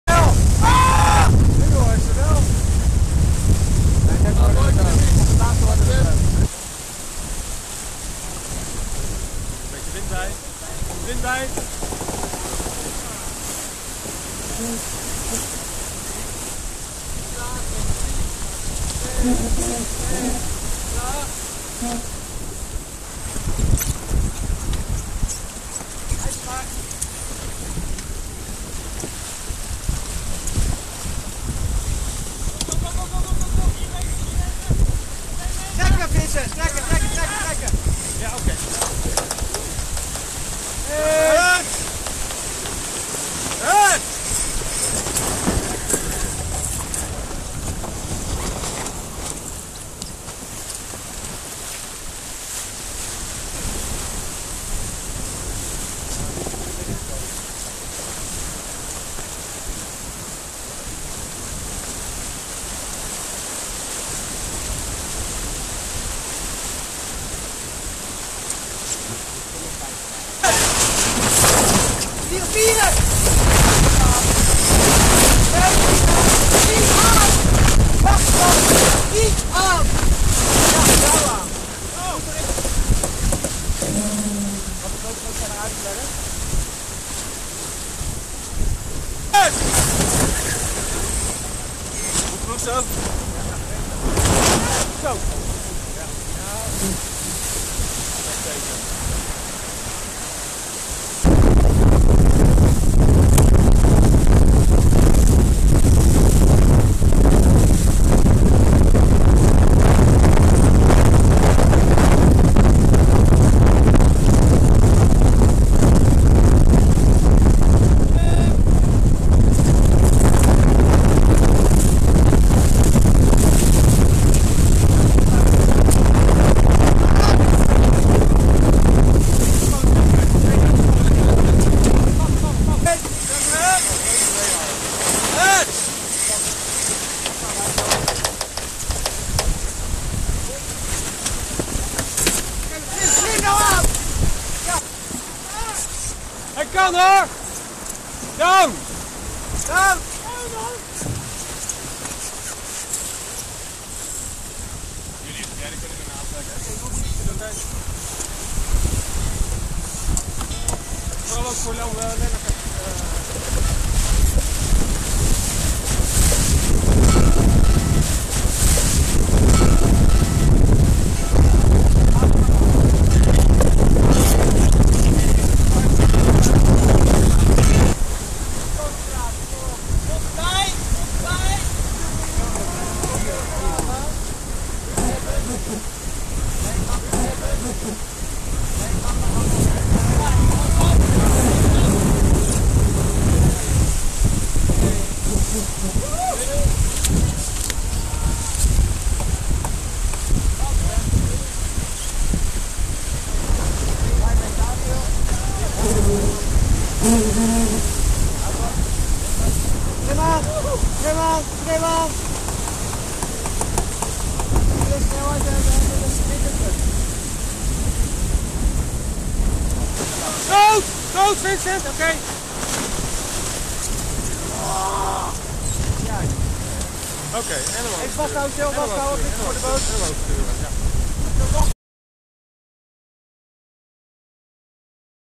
Cowes week 2017 zaterdag 30 juli
Dagje zeilen met lekker wat wind.